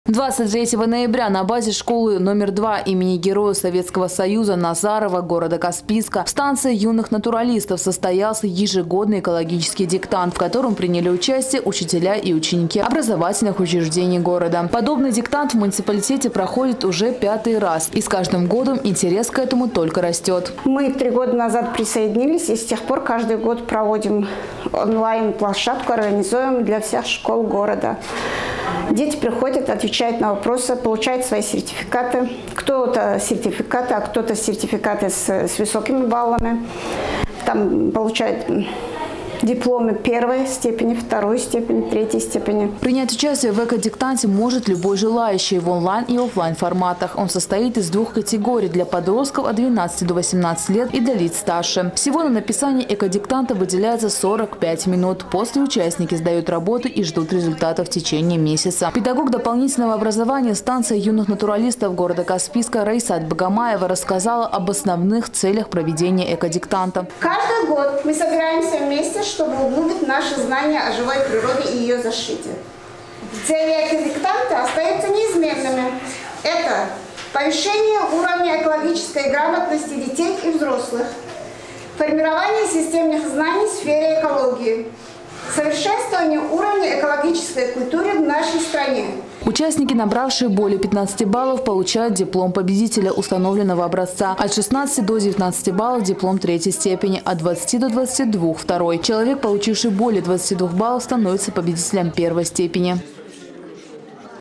В Каспийске состоялся экологический диктант
Пятый год подряд в Каспийске проходит экологический диктант, в котором может принять участие любой желающий, независимо от возраста. В этом году экологический диктант написали более 100 воспитанников всех образовательных учреждений города совместно со своими учителями.